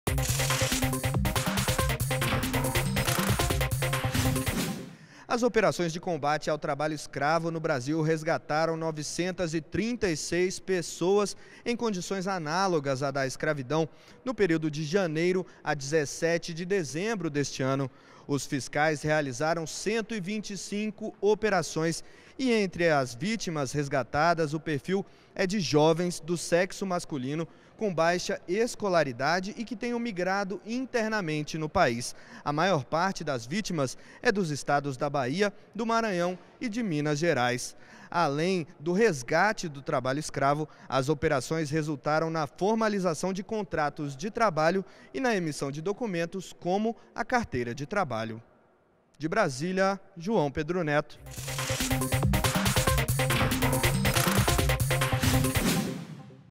Governo resgatou 936 pessoas em condições análogas à escravidão em 2015
ÚLTIMAS - 29.12.15: Um total de 936 pessoas foram resgatadas de condições análogas à escravidão entre janeiro e 17 de dezembro de 2015, segundo balanço das Operações de Combate ao Trabalho Escravo no Brasil. No período, foram realizadas 125 operações. O principal perfil da vítimas é o de jovens do sexo masculino, com baixa escolaridade e que tenham migrado internamente no país.